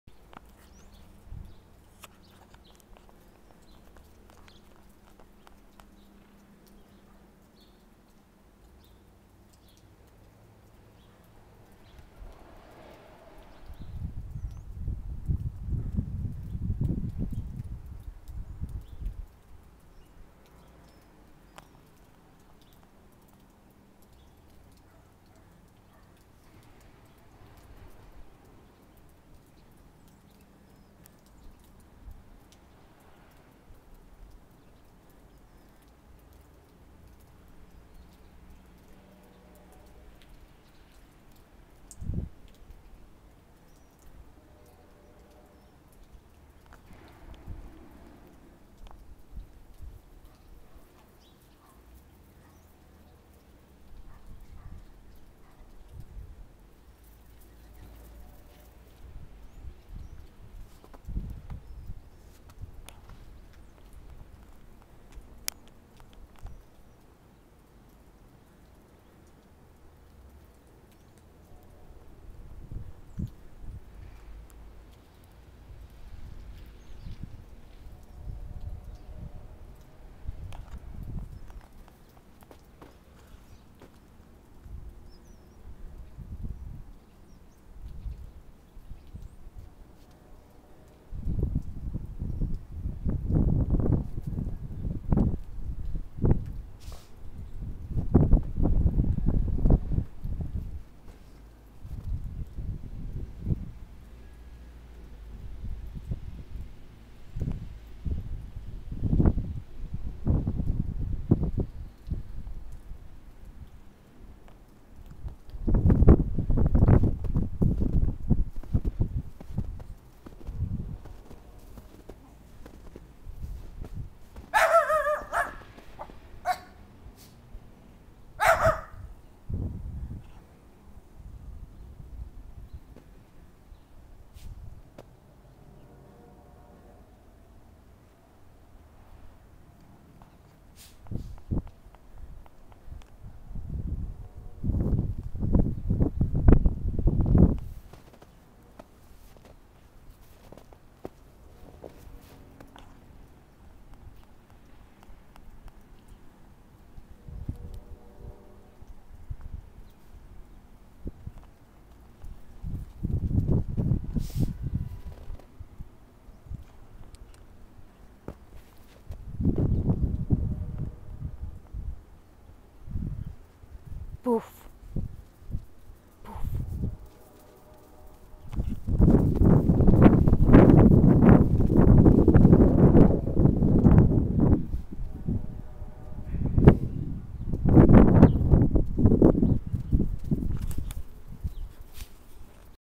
Cotton Schipperkes & Hound: FEBRUARY 9, 2010 COTTON SCHIPPERKES PLAYING IN THE SNOW
Cotton Schipperkes playing in the snow in their backyard, February 9th 2010.